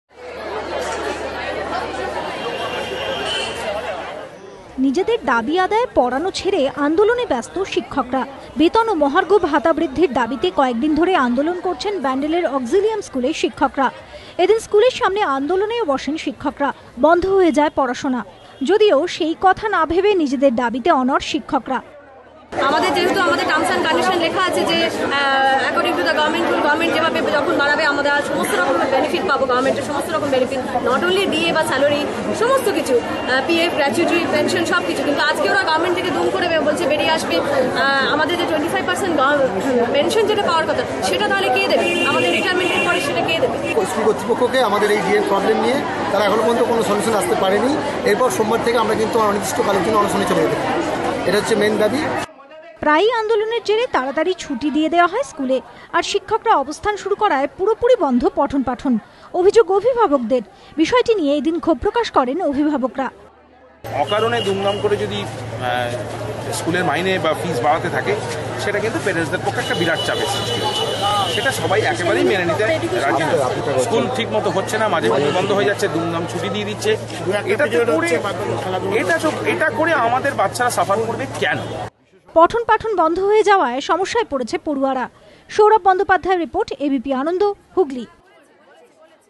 teachers' agitation at bandel school
teachers' agitation at bandel school  resulting in studies being hampered. guardians
disappointed.